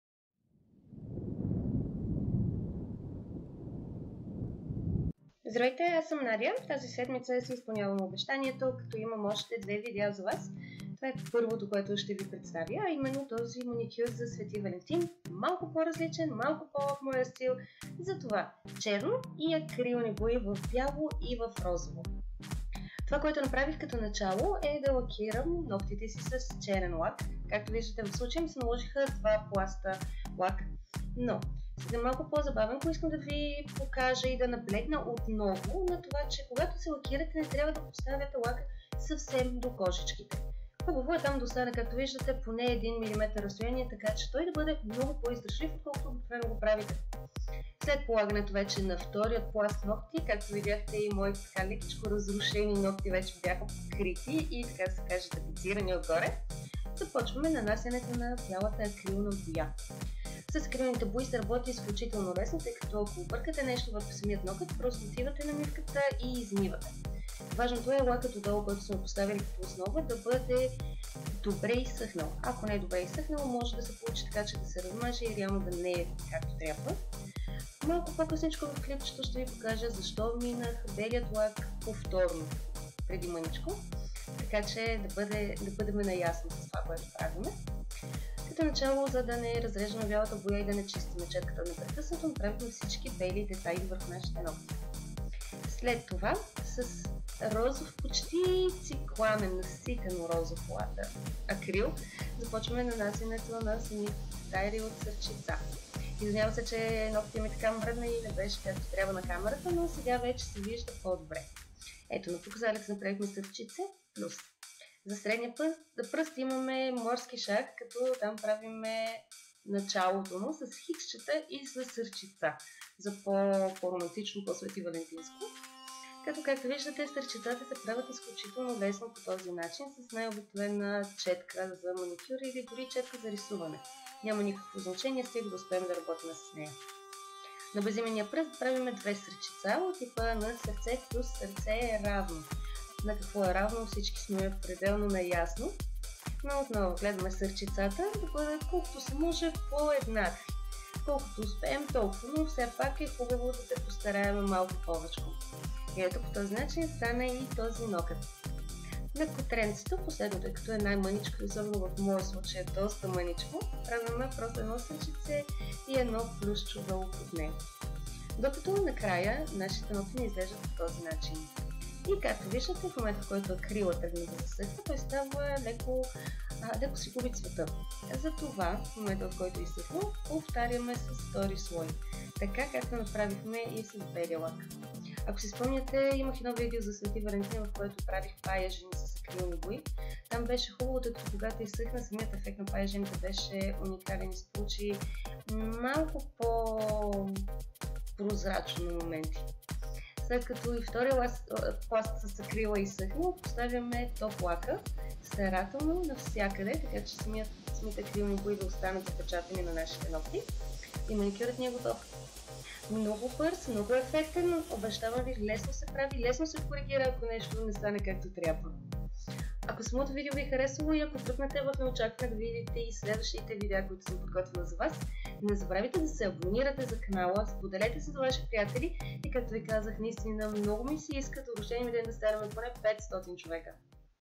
📖💋BTS Different st. Valentine's Day Nail art || Различен романтичен маникюр
Здравейте отново!!! Този път за вас имам различен и мой стил маникюр за романтични поводи. Отново на училище след св. Валентин. Очаквам вашите коментари и предложения в коментарите!

Hello! I'm Nadya and in this channel we can make it all! Just say it!